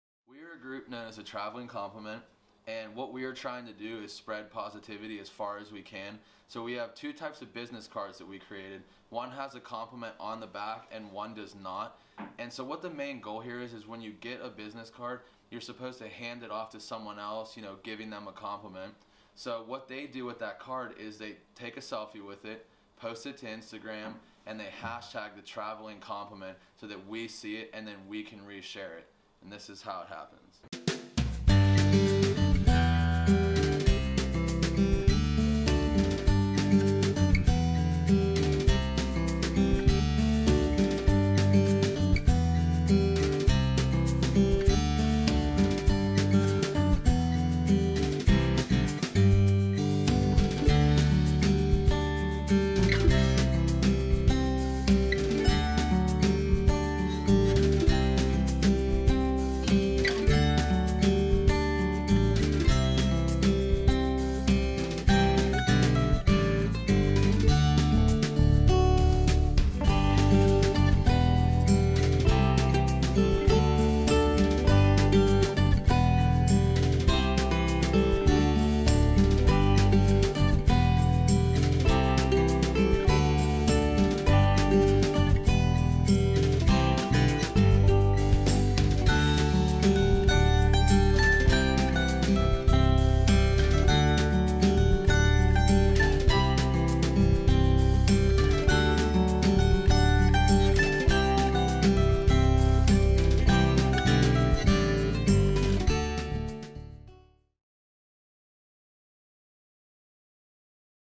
The Traveling Compliment Infomercial2
This is an infomercial on our project that we titled The Traveling Compliment.  Our goal is to spread positivity throughout the world by handing out compliment cards.  Watch our video and see how.